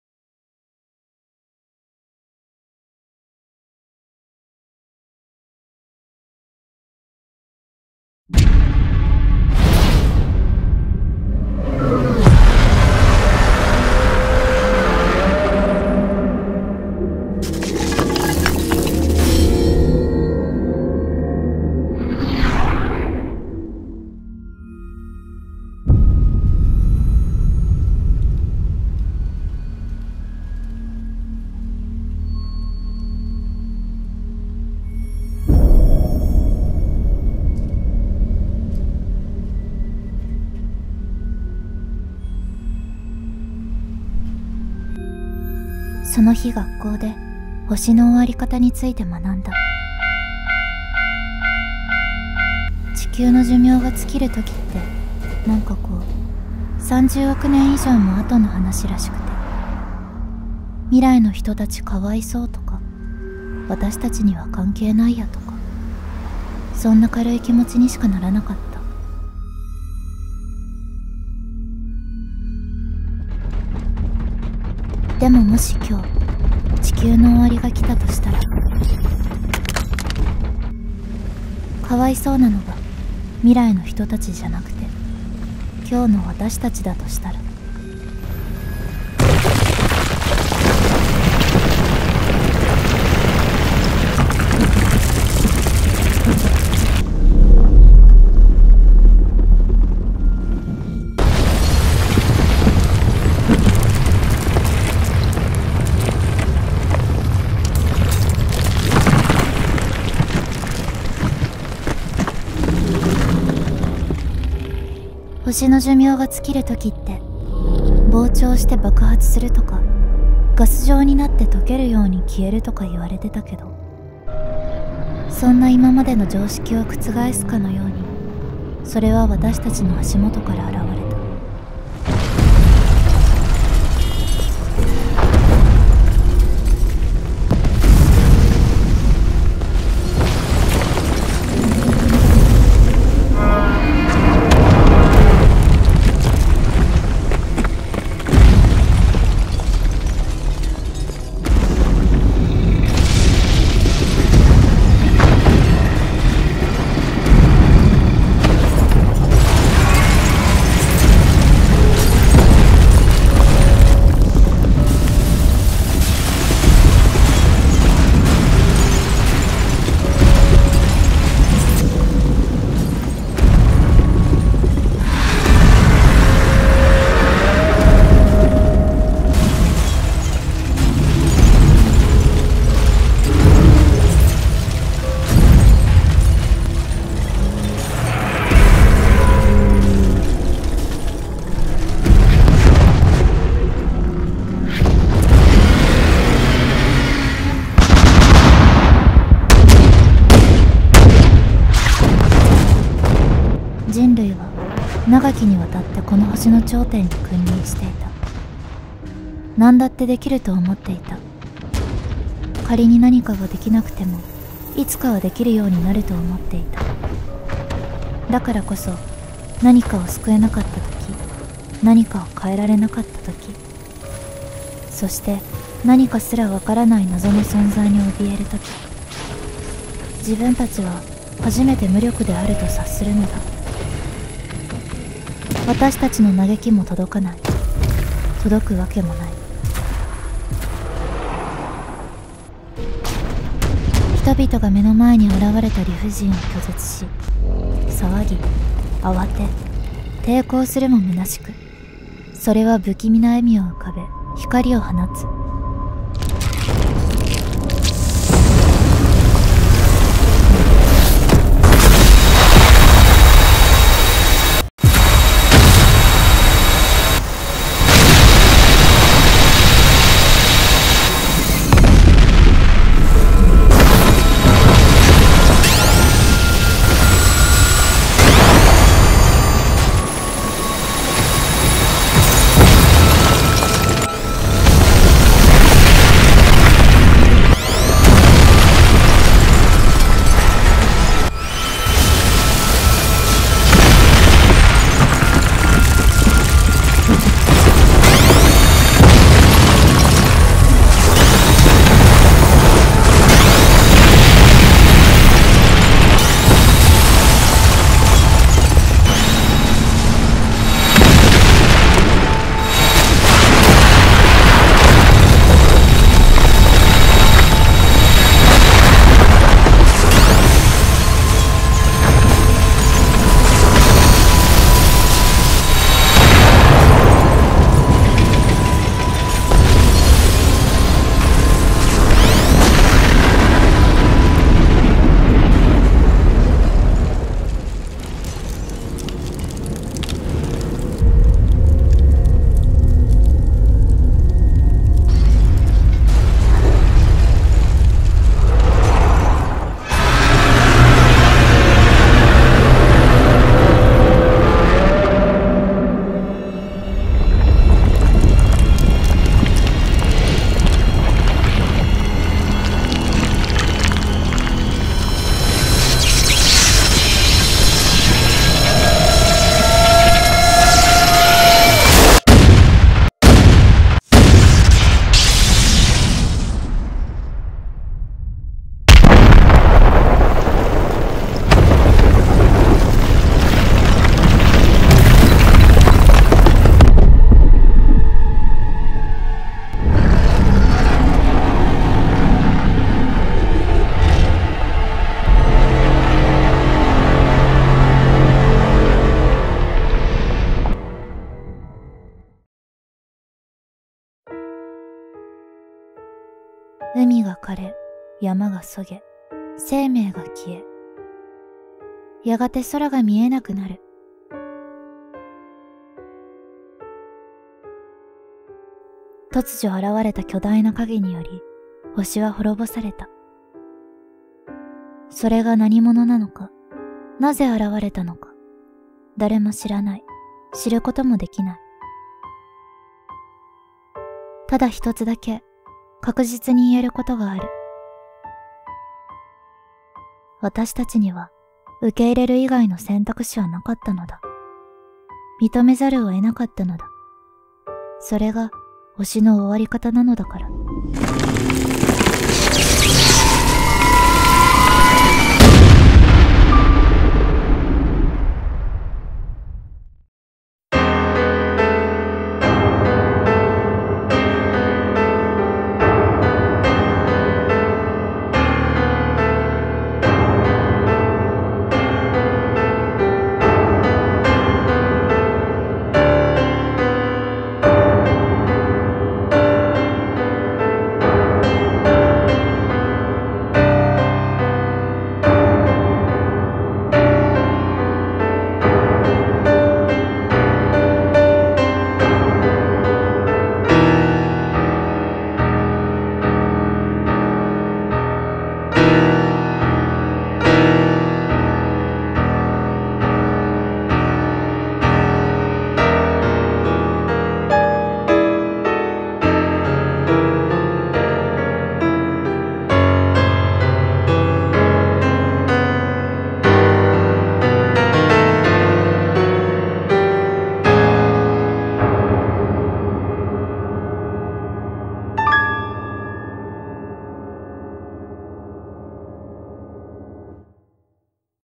【MMD】ゴジラ -死星の刻限-
海が枯れ、山が削げ、生命が消え、やがて空が見えなくなる。

The oceans dry up, the mountains are scraped away, life disappears, and eventually the sky becomes invisible.

クレジットで一か所【】が【」になってますがお気になさらず…

こちら、ByNEETさんのオリジナルデザインのゴジラですが、「ゴジラ・ネメシス」と名付けさせていただきました。

This Godzilla's name is “Godzilla Nemesis.”